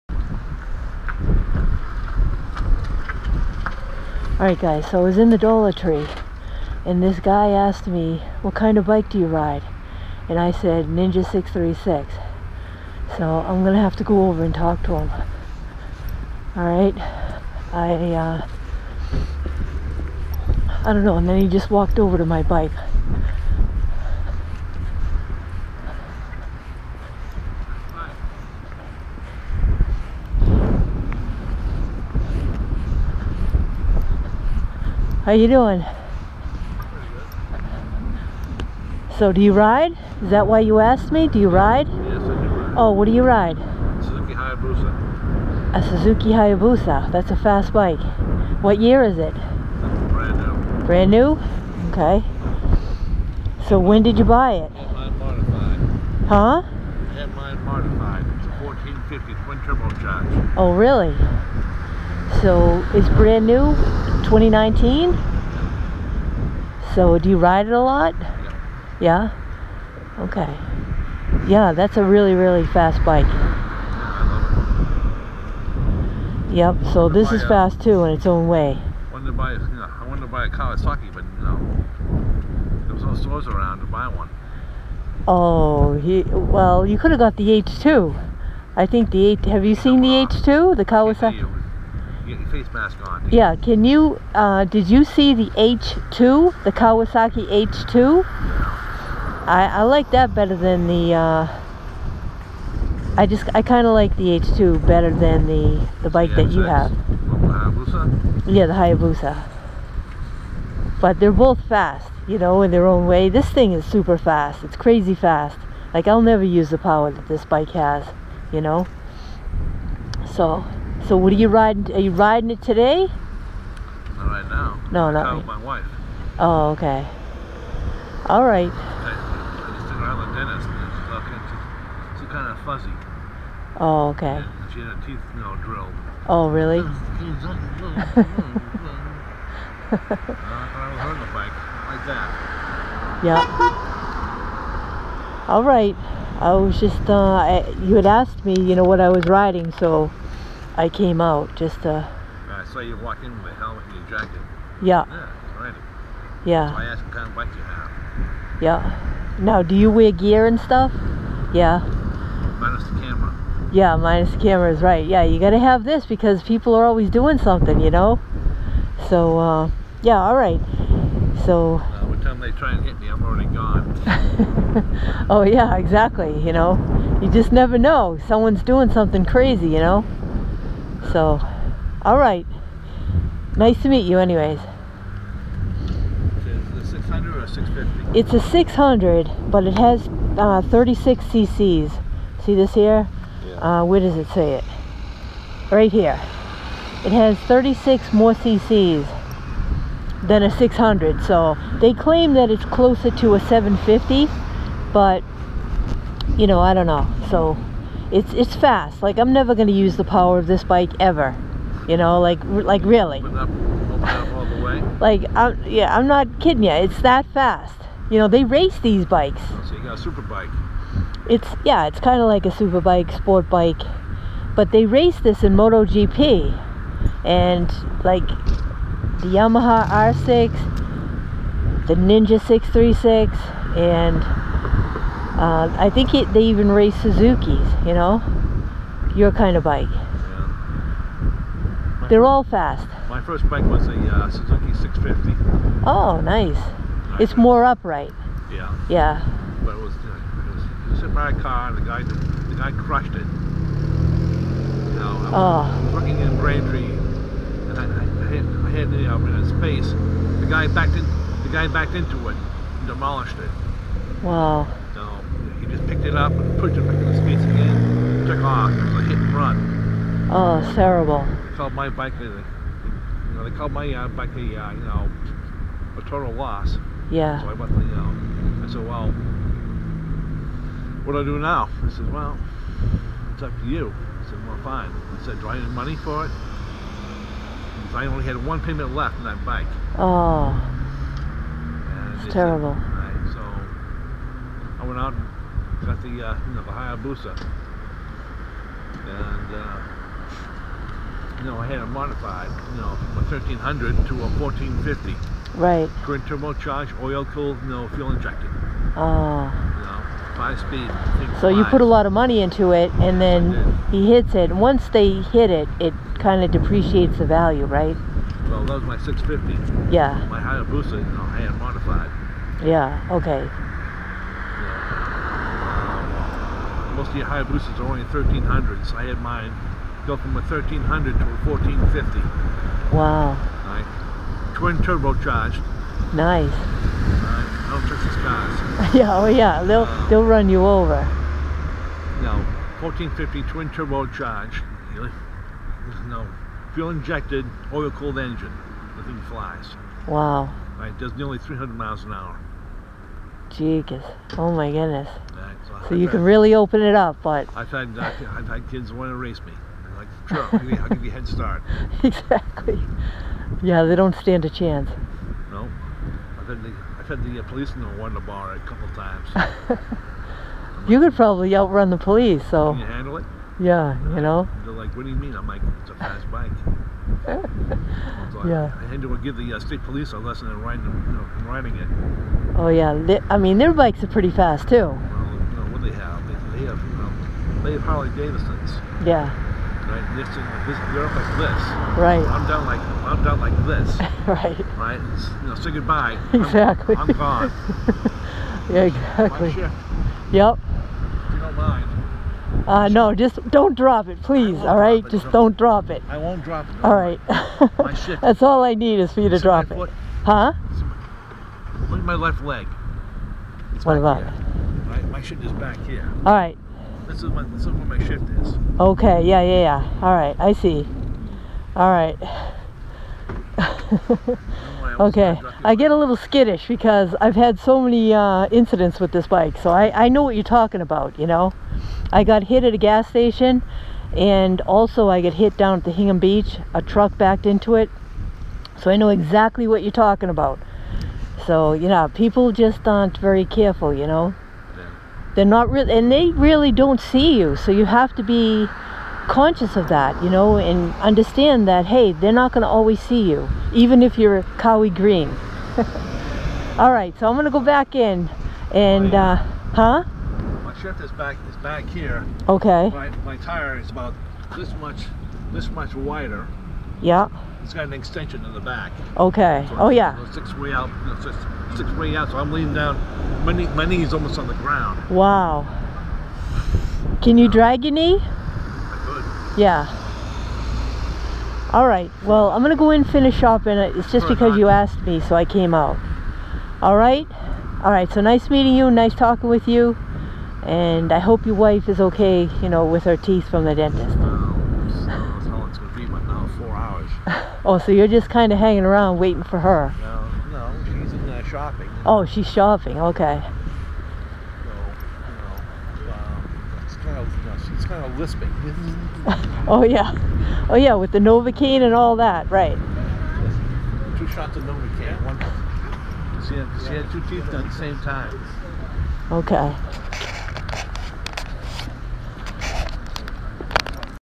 Stranger Sat On My Bike Without Permission!
Filmed on May 15, 2019, I entered the Dollar Tree to buy some snacks for the Survivor Edge of Extinction Finale tonight and this guy in the video approached me inside the Dollar Tree and asked me, "What kind of a bike do you ride?" I was not recording! I told him,  "A Ninja 636!" He said, "Kawasaki?" I said, yeah!" He left the store and walked over to my bike checking out the Ninja 636! I left the store and I walked over to my bike because he was standing near the exhaust on the bike! Then during the conversation, he said, "If you don't mind?" and the next thing I know he's putting his leg over the seat and then he put his foot on the swing arm on the left side of the bike! Usually, I let people sit on the bike, but I was a little apprehensive in this case!